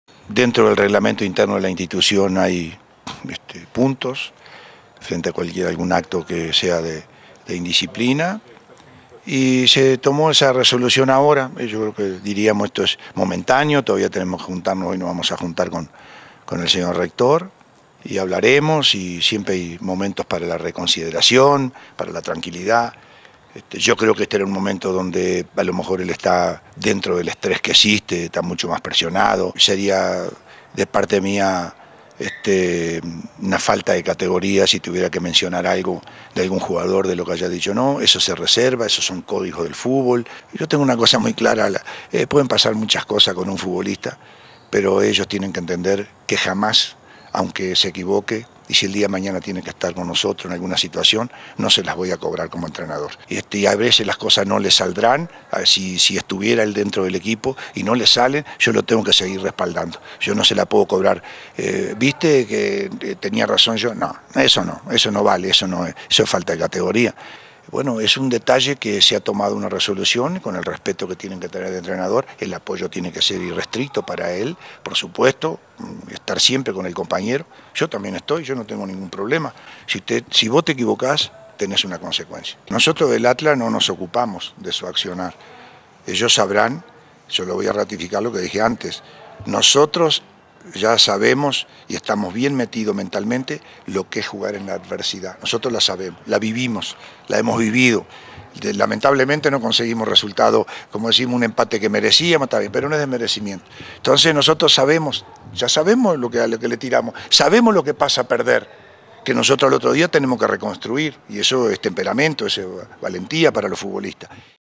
Los errores tienen consecuencias: Héctor Hugo Eugui
CLAUSURA 2012. Zapopan, Jalisco. 20/03/12.  "Si vos te equivocas, vos ténes que aceptar las consecuencias", son las palabras que utilizó el timonel de Estudiantes al cuestionarle acerca de  la sanción  impuesta al jugador Rubens Sambueza.
 Fue en el cotejo contra Morelia, que  Sambueza  externo su molestia ante las decisiones de   Héctor Eugui, que más tarde desembocarían en reclamos durante el entrenamiento, por tal indisciplina, el jugador fue separado del primer equipo para entrenar ahora con la selección SUB 20 de la escuadra Zapopana.
Eugui, señalo que la resolución es momentánea y que Rubens sigue teniendo todo el apoyo de la directiva así como también del cuerpo técnico. Agrego también que no hará declaraciones acerca del intercambio de palabras, pues hacerlo, rompe con el código futbolístico.